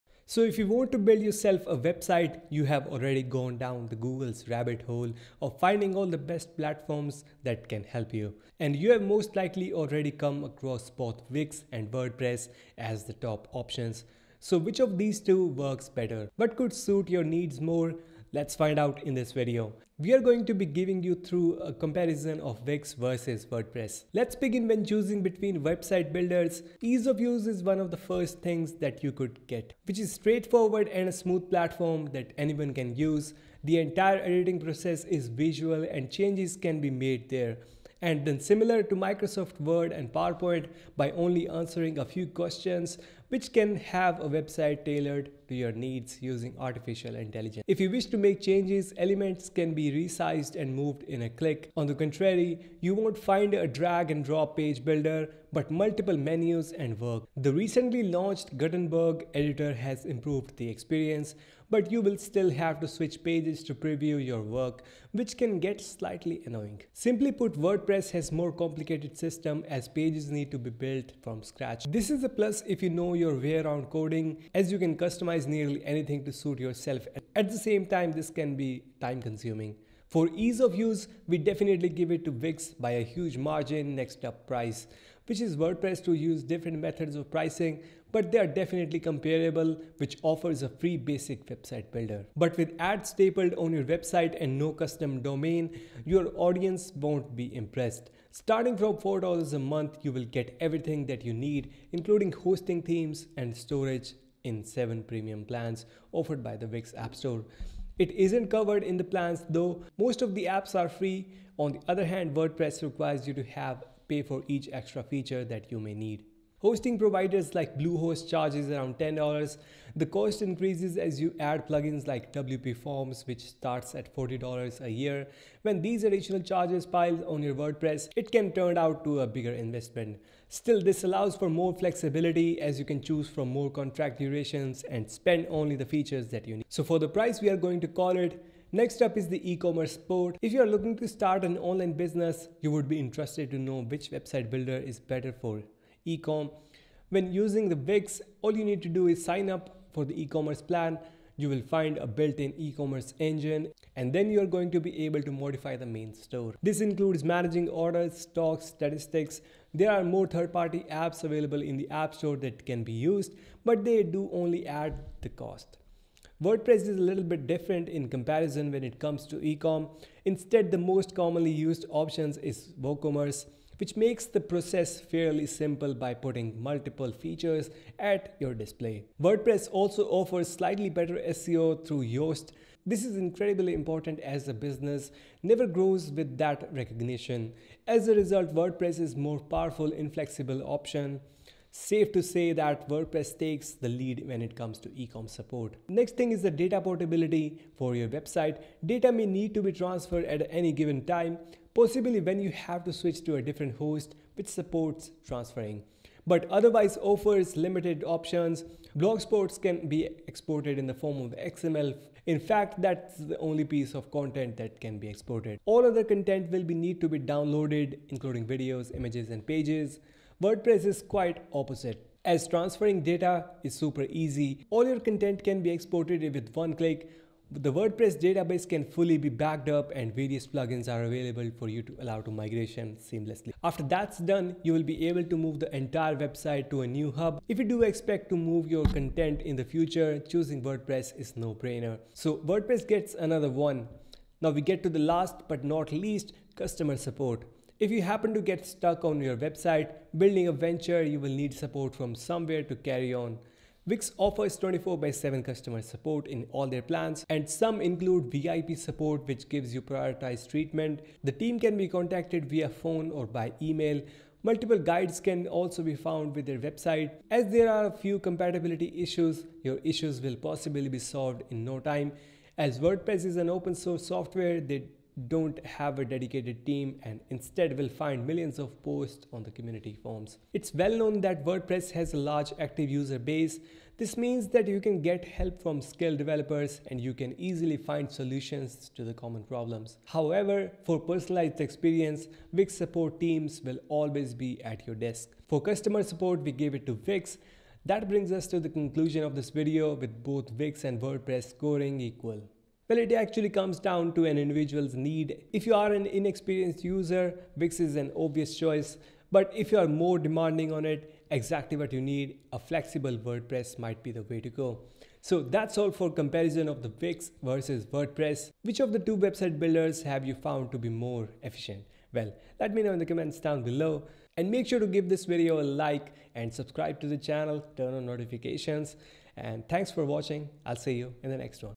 Wix Vs Wordpress 2022 | Which One Should You Prefer?
00:00 - Intro
00:10 - Wix vs Wordpress
01:24 - Pricing
01:56 - Hosting
02:21 - Ecommerce
03:15 - Migration
04:15 - Customer Support

*Kindly ignore tags mentioned below*